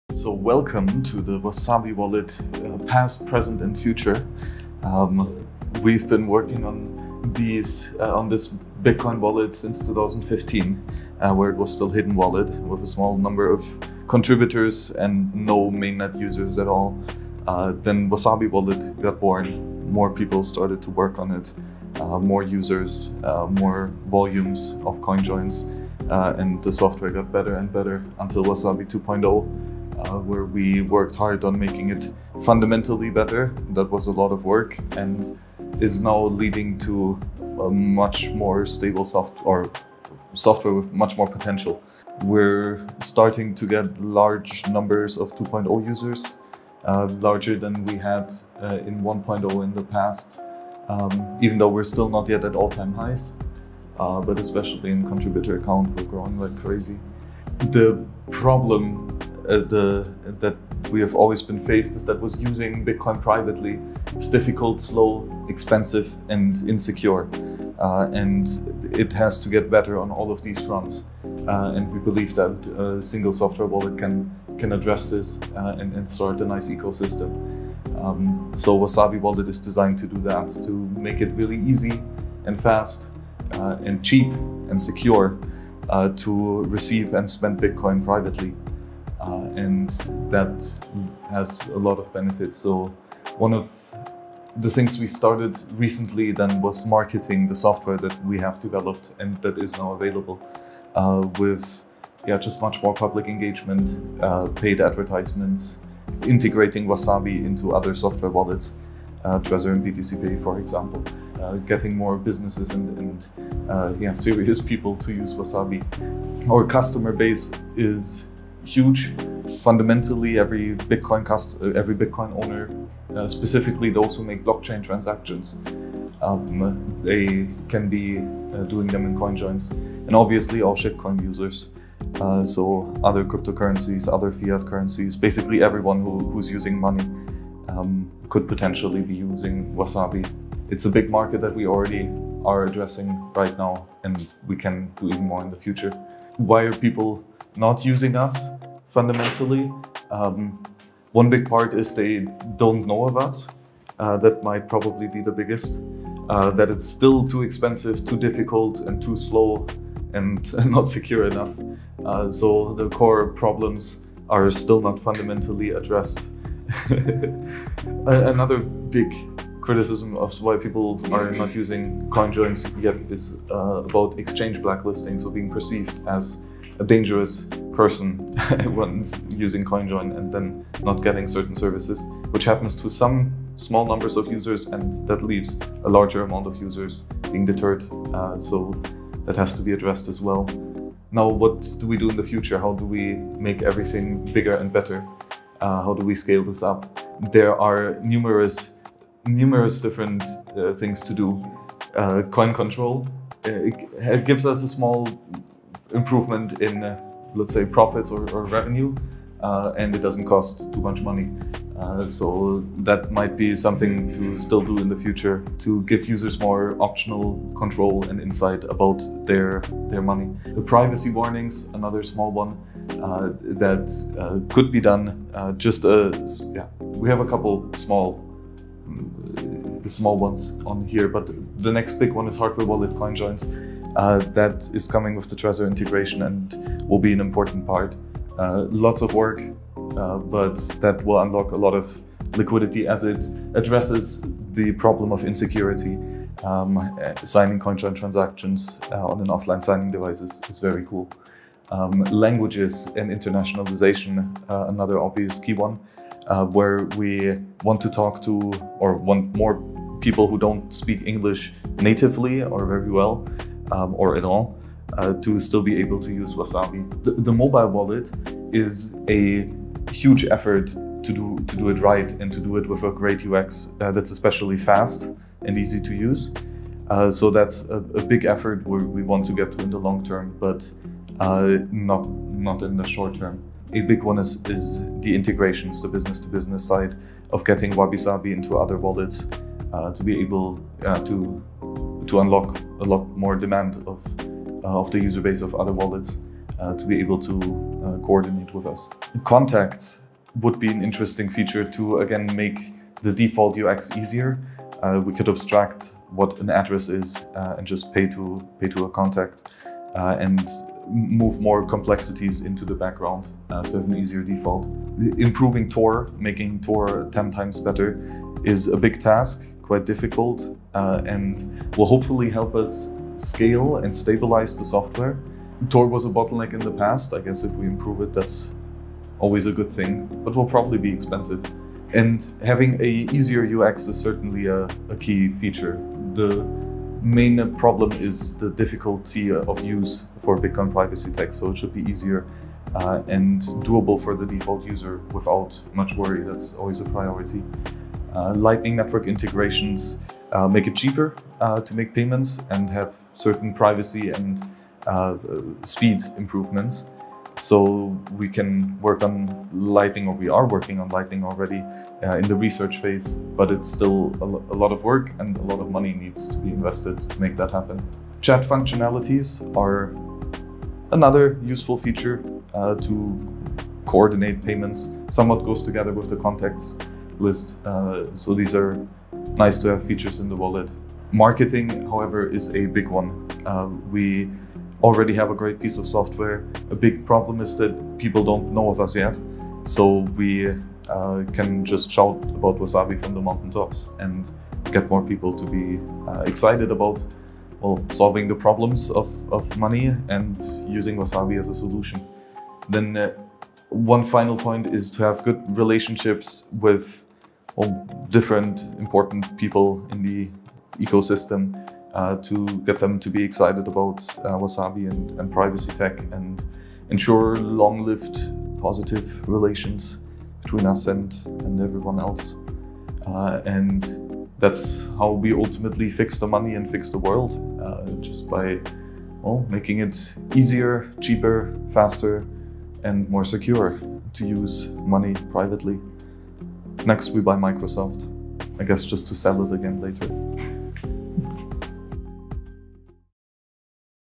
Wasabi Wallet | Past, Present & Future
Catch up with Max, on the coming plans and goals for Wasabi.